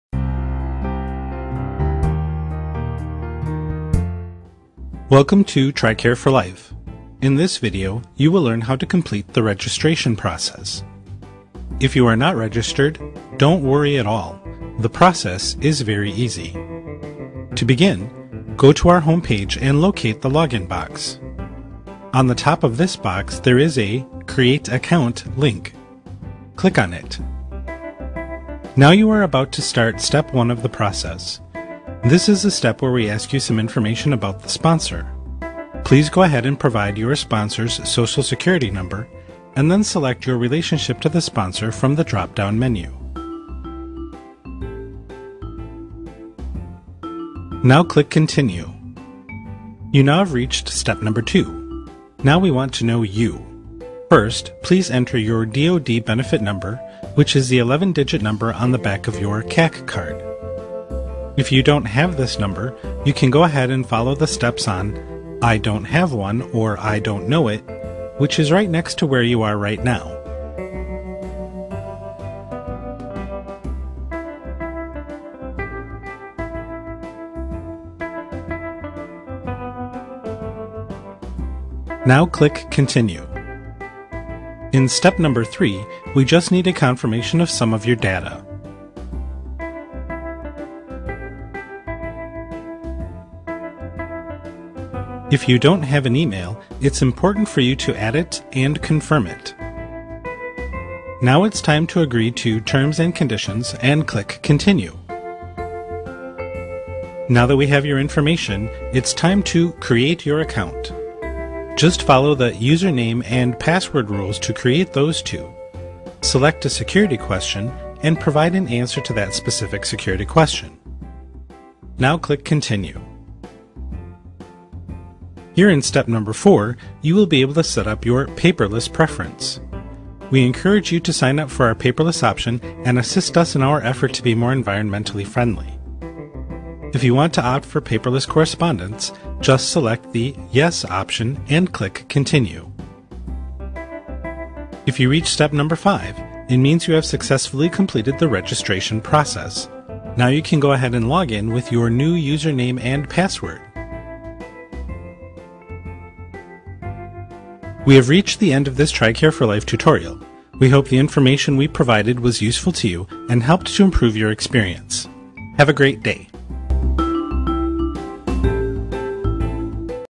TRICARE for Life- Registration Tutorial
Instructional video on on how to create an account on TRICARE4u.com.